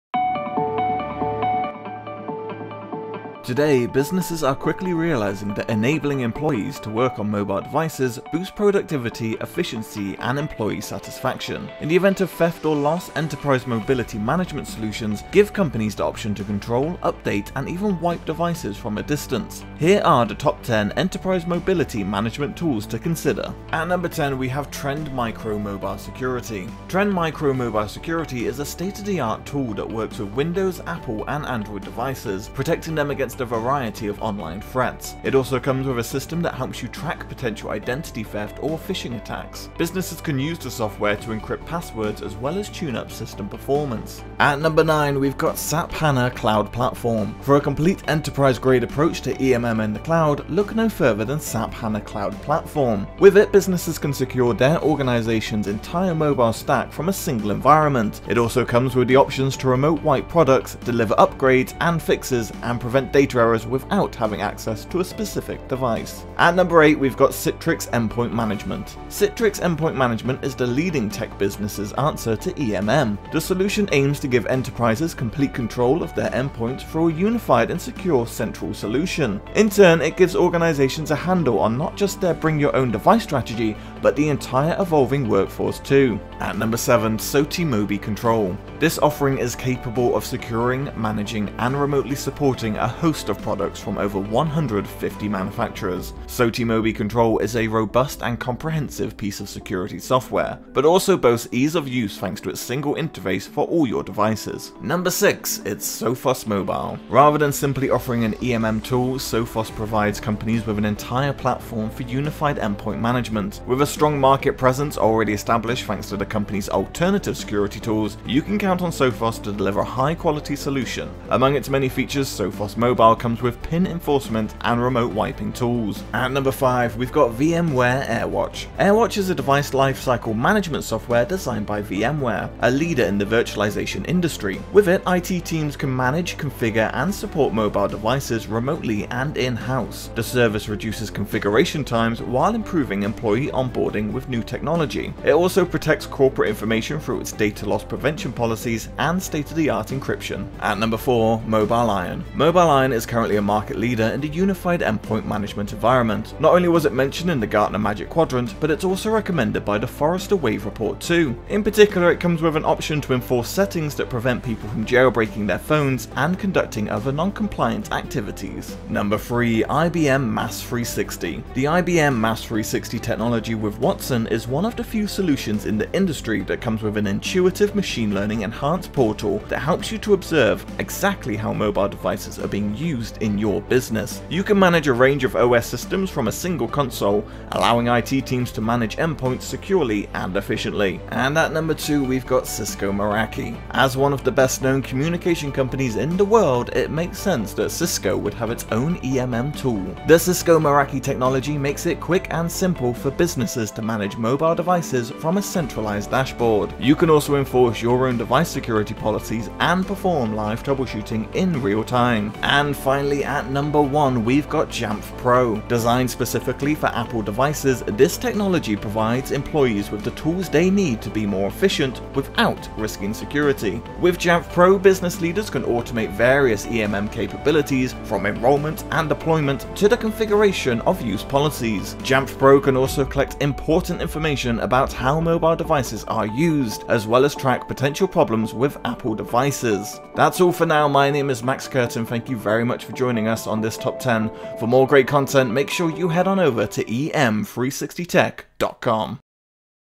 Top 10 Enterprise Mobility Management Solutions
Enterprise mobility management (EMM) solutions give companies the option to control, update, and even wipe devices from a distance. So, which EMM tools are leading the way today?